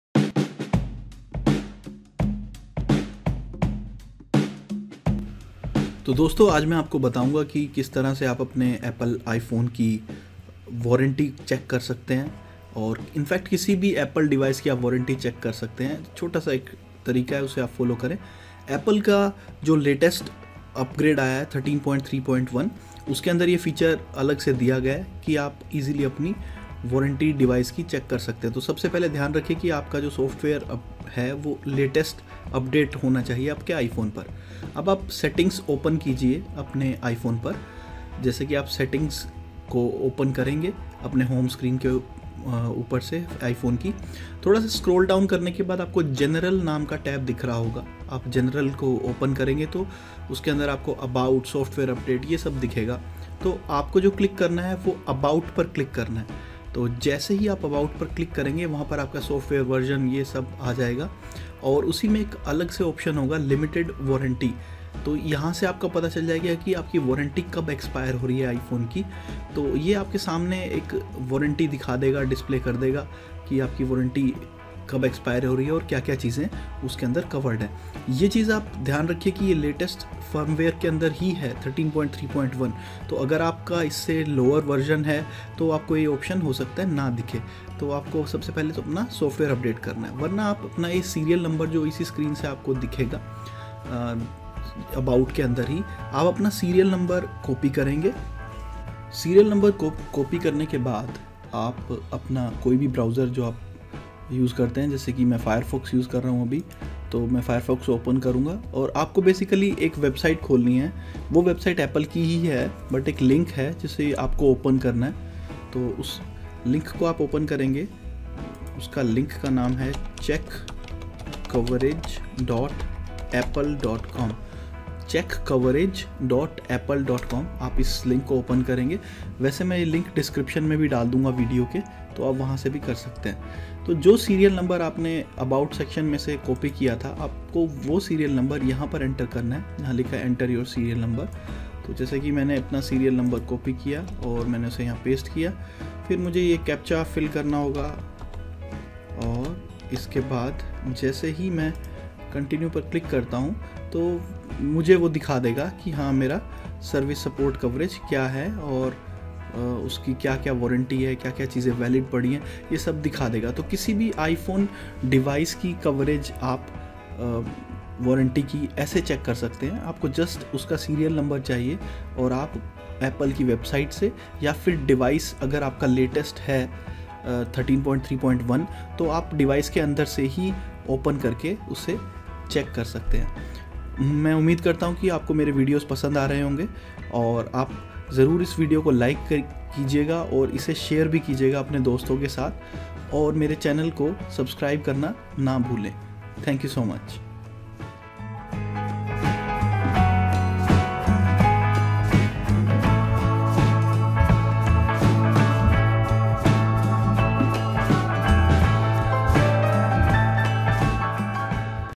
How to check warranty of any Apple iPhone? Explanation in Hindi.
This is a quick explanation on How you can check the warranty of your iPhone or Apple device. You can check the warranty of your iPhone through the in-built settings option or you can copy the serial number of your mobile device and check the warranty from the Apple website.

To check the warranty of your iPhone, you would need to have the latest version of iOS (13.3.1 as on today) in your iPhone. If you have a lower vbersion, you may still check the warranty by visiting and pasting the serial number there.